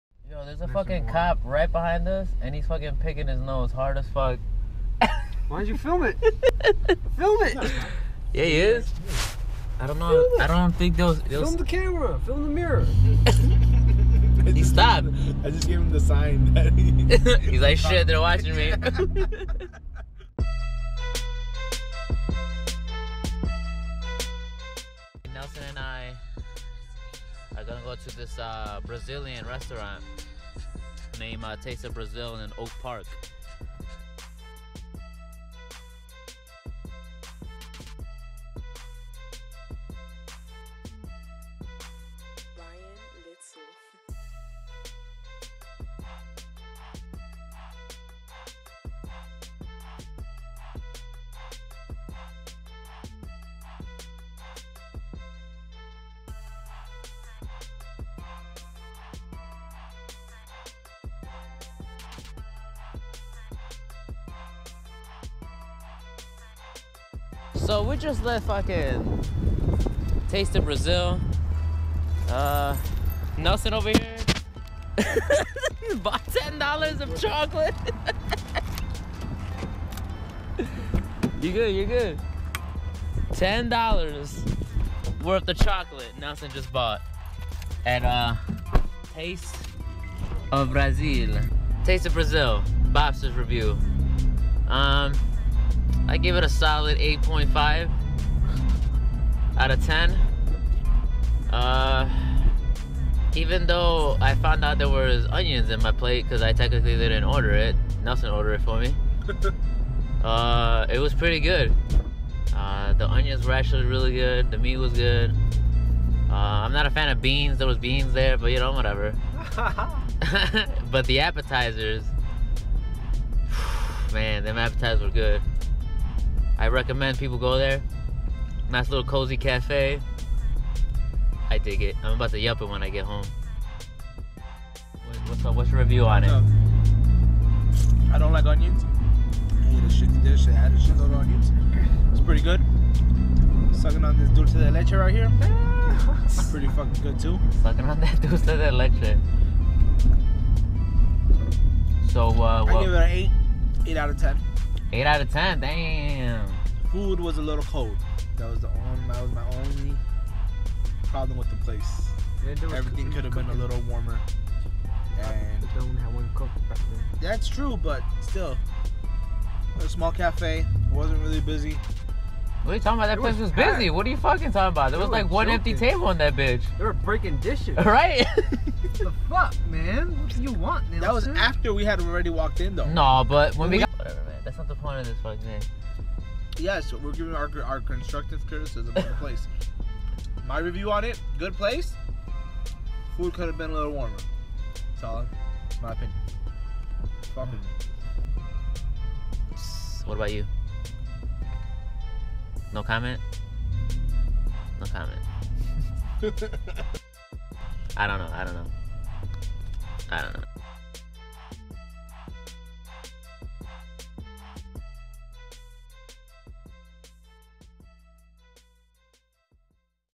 Especially Delicious : Taste of Brasil
On our very first episode Nelson and I decided to check out Taste of Brasil. We both heard good things so we took a ride and went get food. Enjoy!

Music is produce from the one and only
Ryan Little! Make sure you check out his channel!